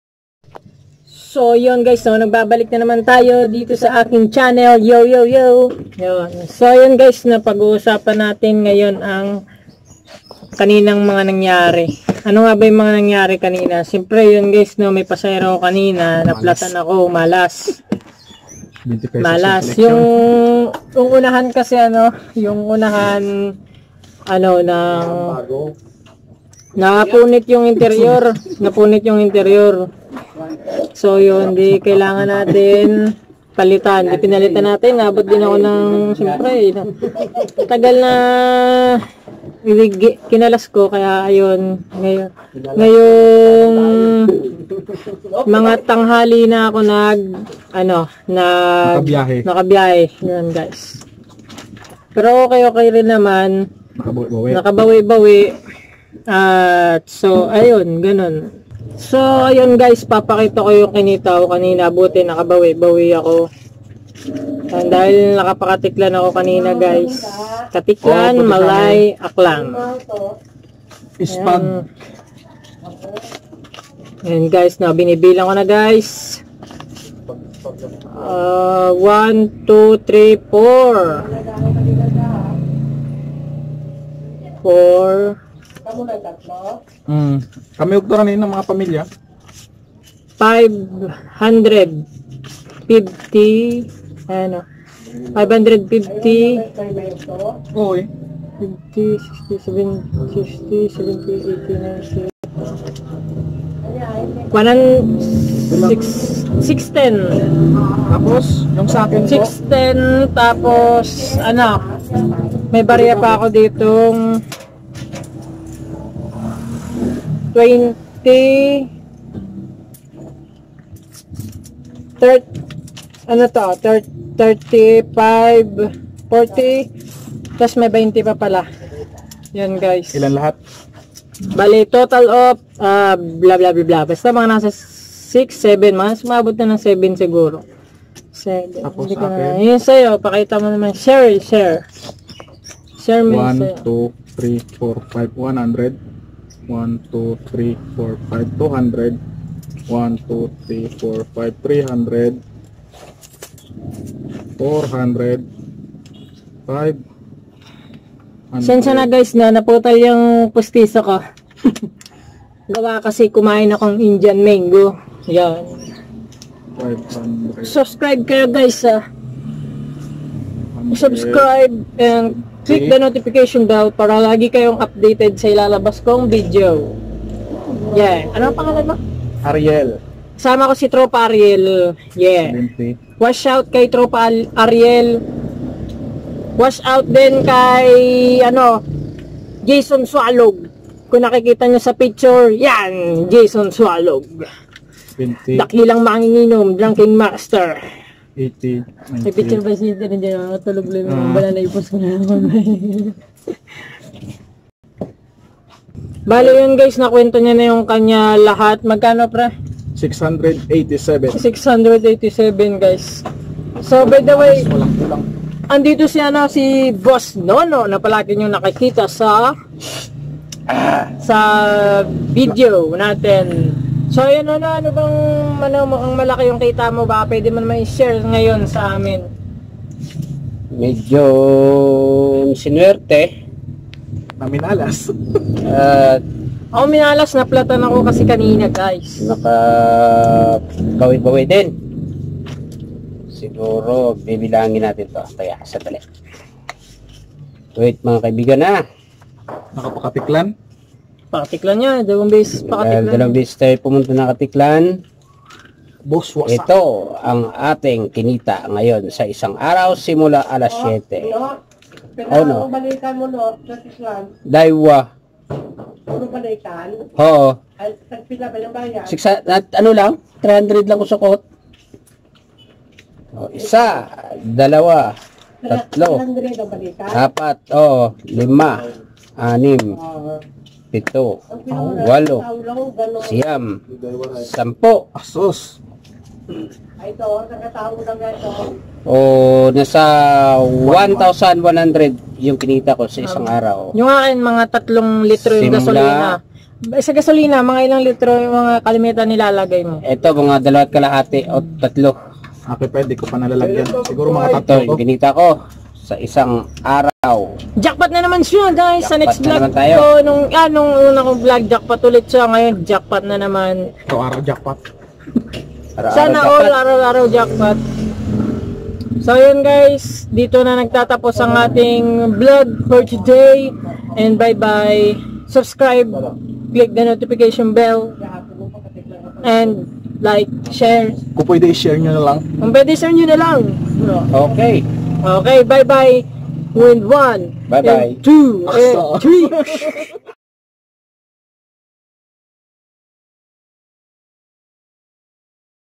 Na platan ako kanina
kaninang umaga bigla nalang napunit ang interior
kaya pinaayos ko muna sa madaling salita pinalitan.
ng interior.